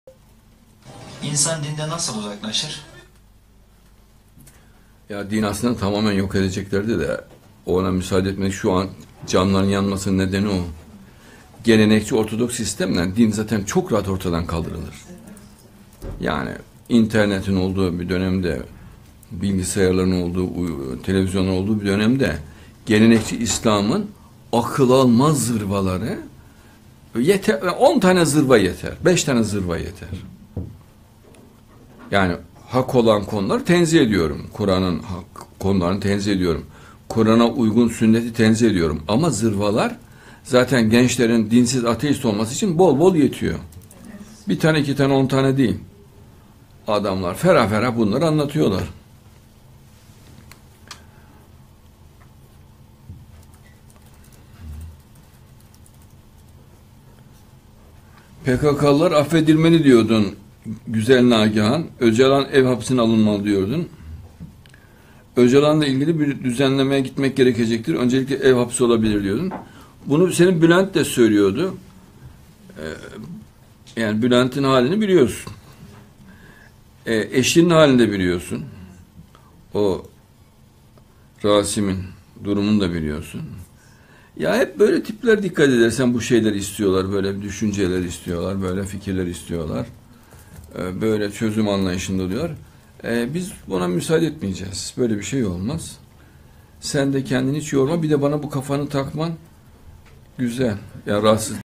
İNSAN "dinden nasıl uzaklaşır?"
Duygusallığa ve şirke kayıldığında iman zedelenir. Müslümanın akıllı iman derecesini hiçbir zaman bırakmaması gerekir. Allah şirke kayılmasından razı olmaz. “Benim moralim bozuk” demek, Allah’ın her şeyi hayırla yarattığını unutmak demektir. Bu şirk mantığından şiddetle uzaklaşmak, Allah’a sığınmak gerekir. (A9TV, 29 Ekim 2017)